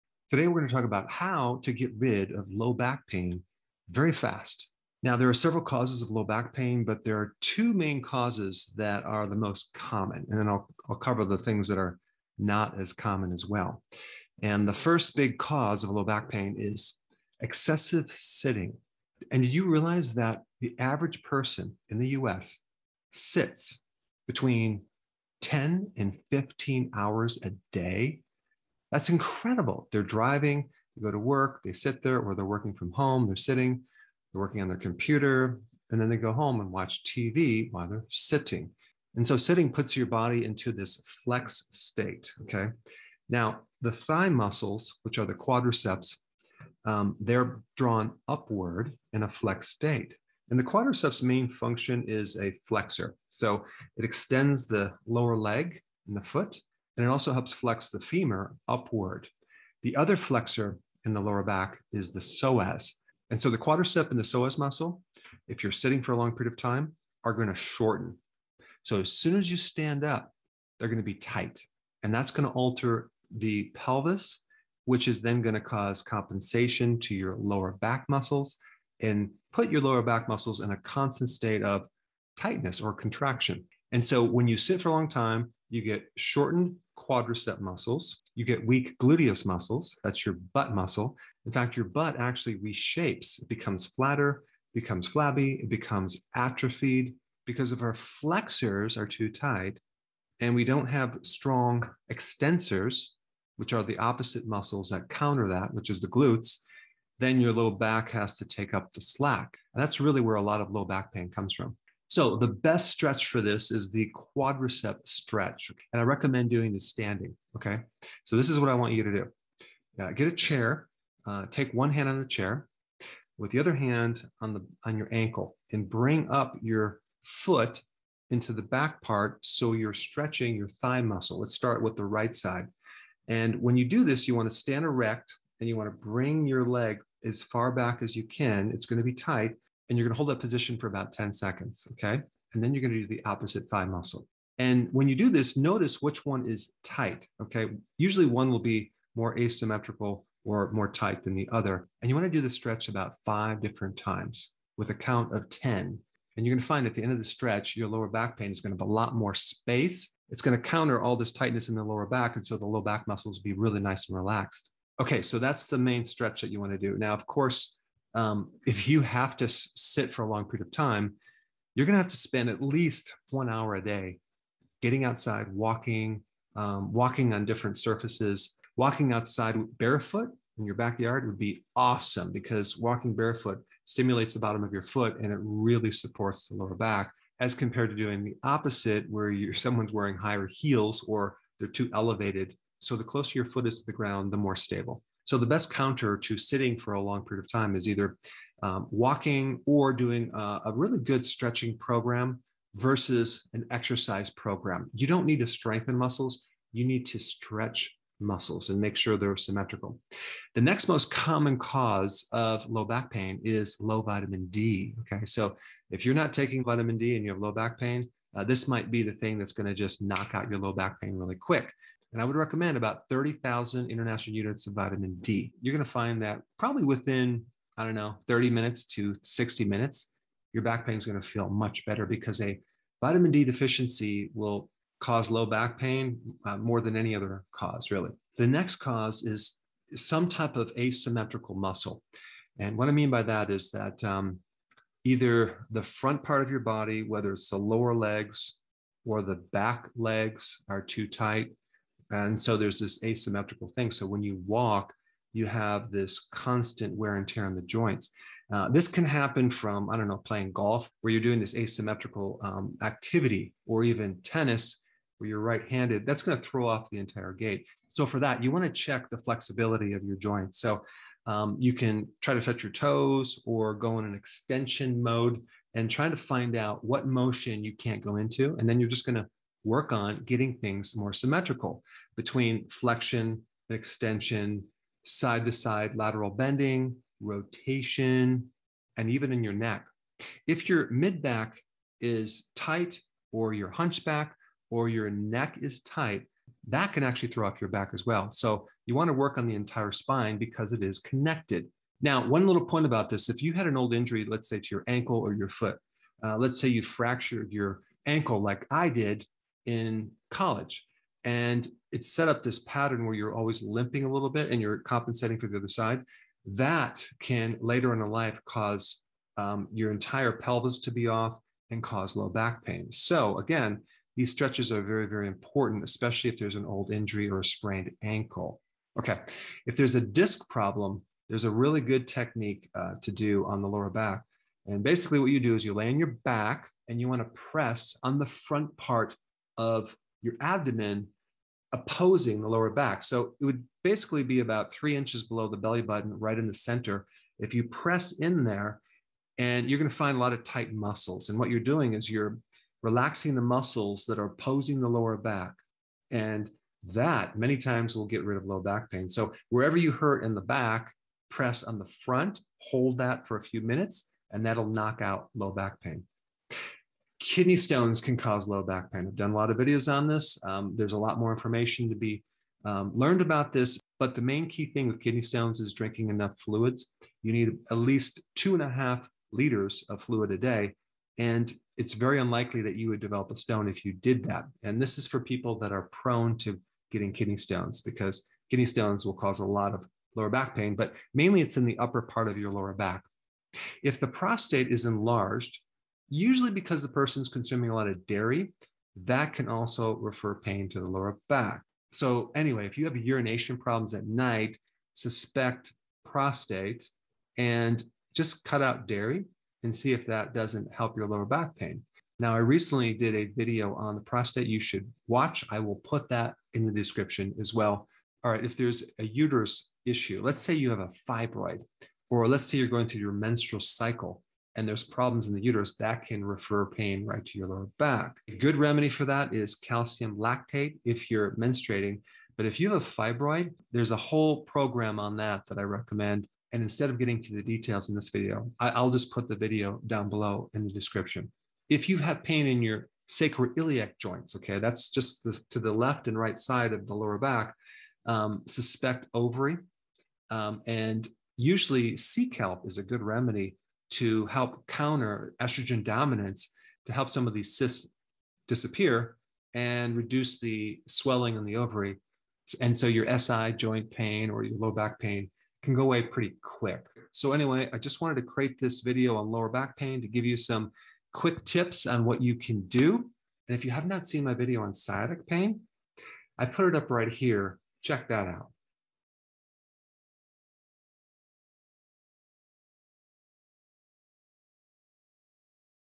How to Fix Your Low Back Pain (INSTANTLY!) - Dr. Berg
Find out what causes low back pain and how to fix low back pain fast!

0:00 Introduction: Fix low back pain fast
0:15 #1 Excessive sitting
3:30 #2 Low vitamin D
4:00 #3 An asymmetrical muscle
5:30 #4 A disc problem
6:07 #5 Kidney stones
6:33 #6 Enlarged prostate
7:00 #7 Problems with the uterus
7:22 #8 Pain in the SI joint
7:50 Check out my video on sciatic pain!

Today we’re going to talk about how to get rid of low back pain fast.

Low back pain causes and remedies:
1. Excessive sitting
What you can do:
• The quadriceps stretch
• Spend at least 1-hour walking

2. Low vitamin D
• Take vitamin D (about 30,000 IU)

3. An asymmetrical muscle
• Pinpoint the motion you can't go into, and use stretches to work on it

4. A disc problem
•  Lay on your back and press on the front part of your abdomen opposing the pain in your lower back

5. Kidney stones
• Drink at least 2.5 liters of fluid a day

6. Enlarged prostate
• Cut out dairy

7. Problems with the uterus
• Take calcium lactate (during menstruation)

8. Pain in the SI joint
• Consume sea kelp

I hope this helps explain how to fix low back pain fast.